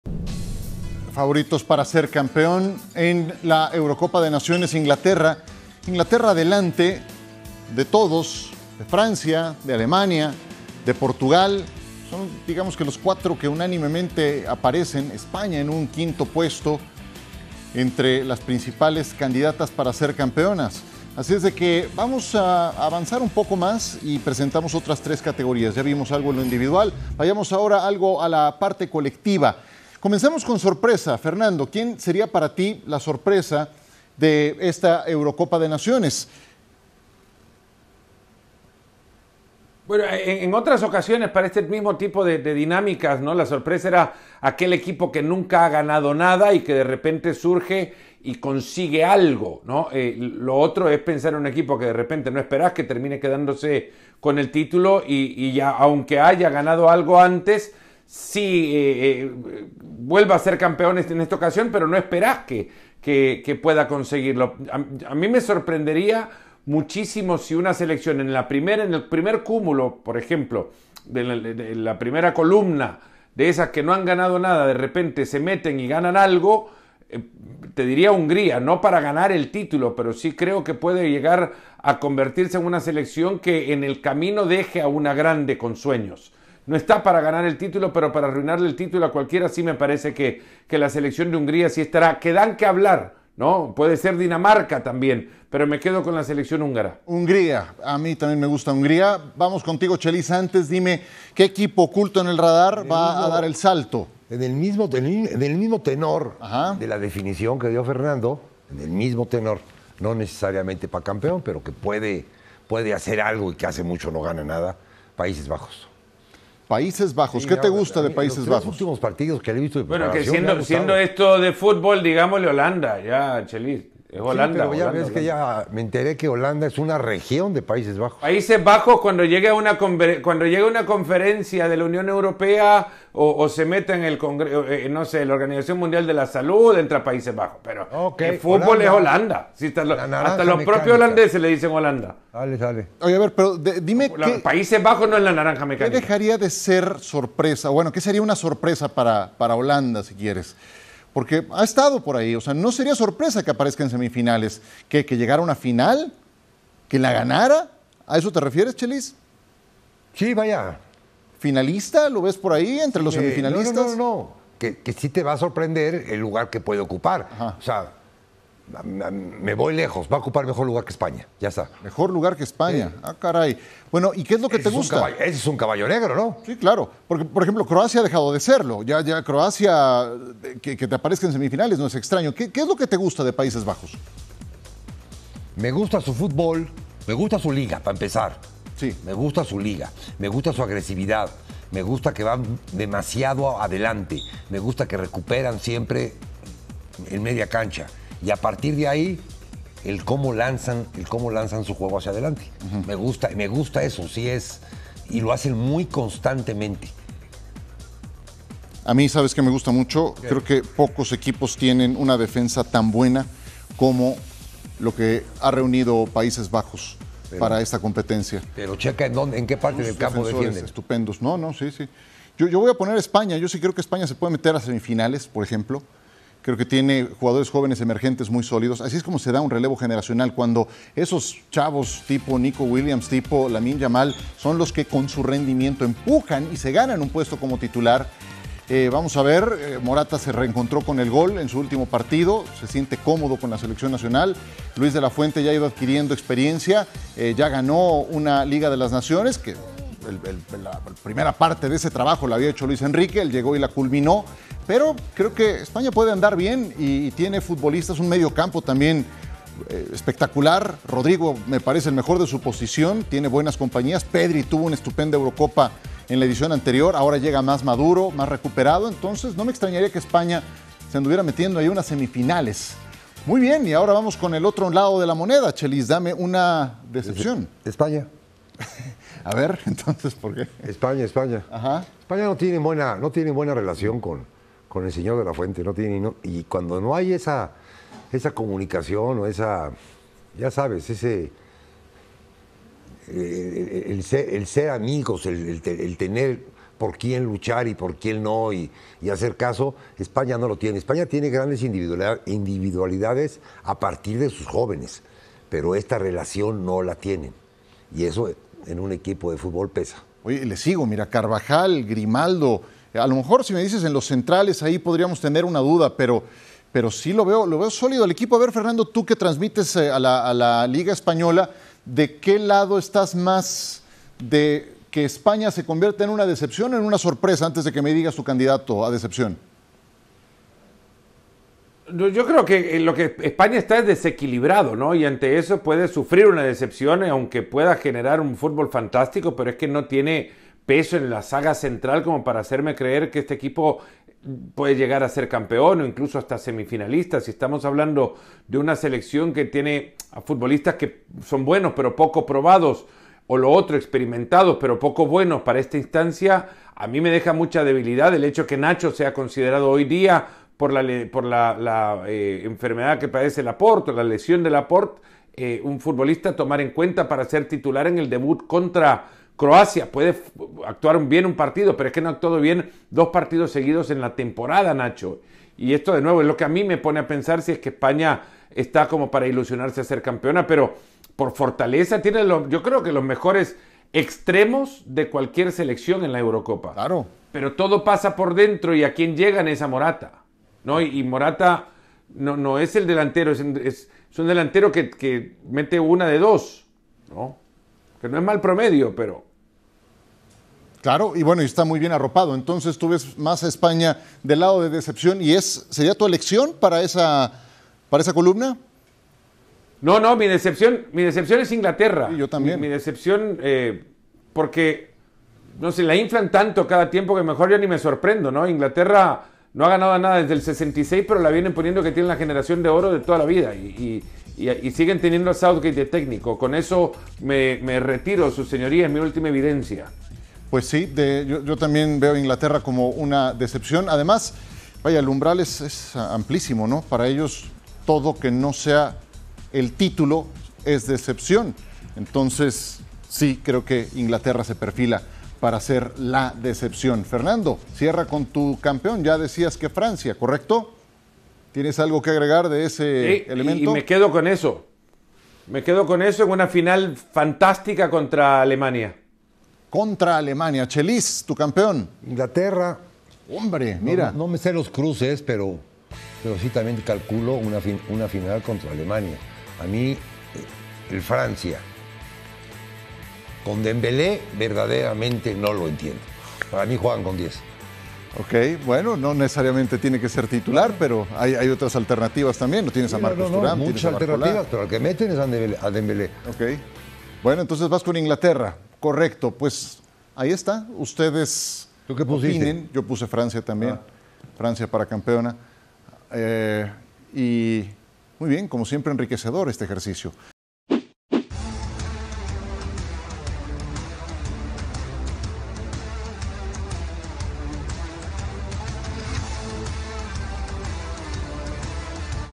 FRANCIA, campeona. INGLATERRA, decepción. ESPAÑA, HUNGRÍA y PAÍSES BAJOS sorpresa en EURO | ESPN FC
En ESPN FC, hacen sus pronóstica de la Eurocopa 2024 donde colocan a la selección de Francia como la campeona del torneo, Inglaterra como la decepción. Mientras que España, Hungría y Pasíses Bajos pueden dar la sorpresa. espnfc eurocopa francia | ESPN Deportes